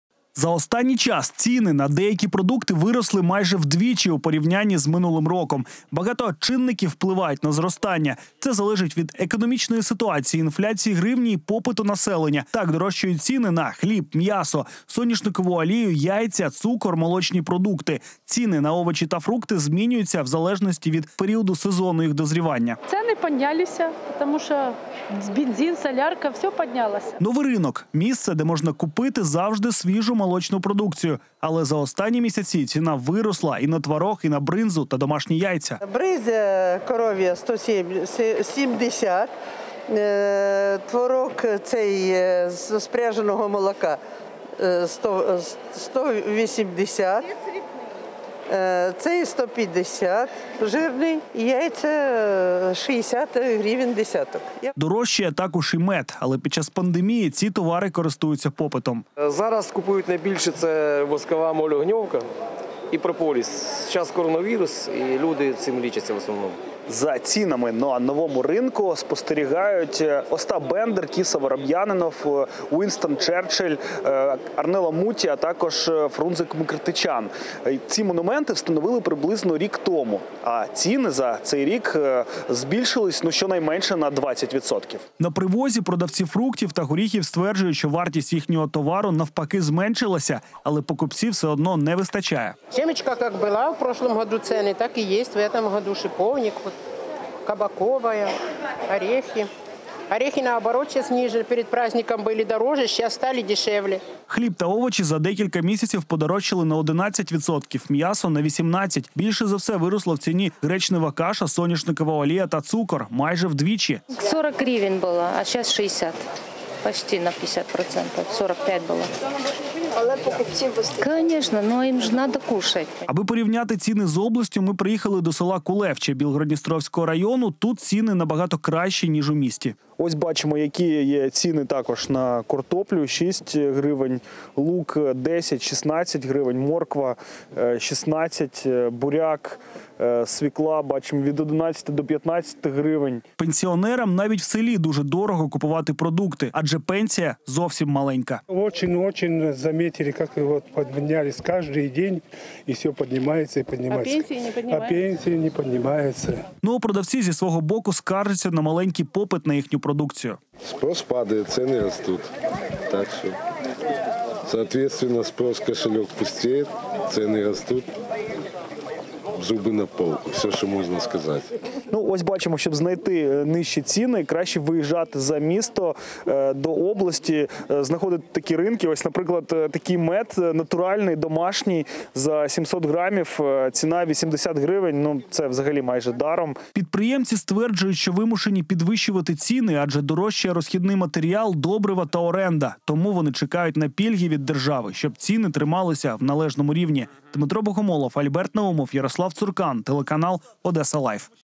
Ціни на продукти зросли вдвічі? Інспекція одеських ринків
Ведучий оперативного штабу Дмитро Богомолов вирушив на одеські ринки, аби дізнатися актуальні ціни на продукти та на чому можна заощадити.